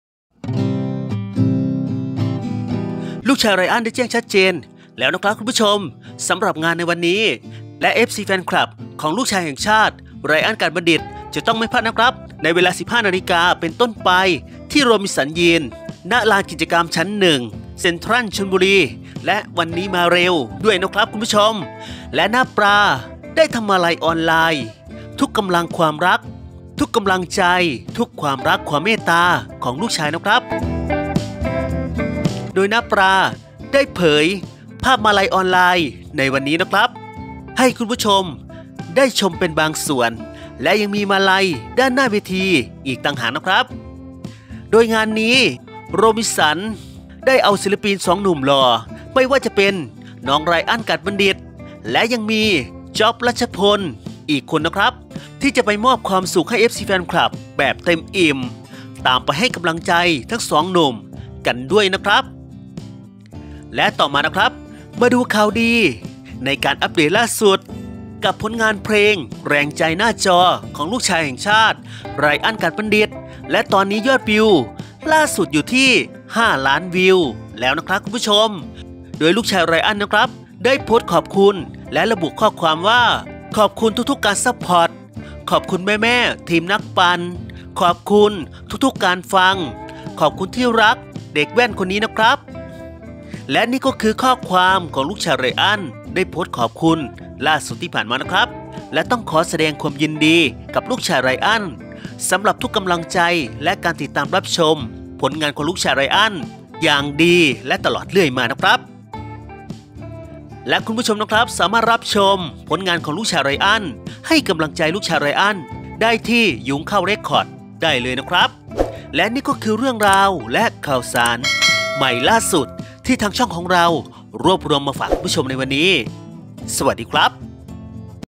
ไรอัล บอกอะไรแฟนคลับ
ไรอัล ฝากขอบคุณและแจ้งคิวงานในวันนี้ที่ชลบุรี